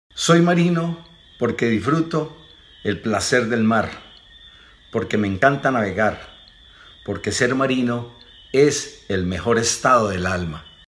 SoyMarinoPorque
Únanse a la campaña SoyMarinoPorque y hagámosle un homenaje a los mares que observan nuestras más altas montañas y a los ríos que llevan nuestros sueños al mar.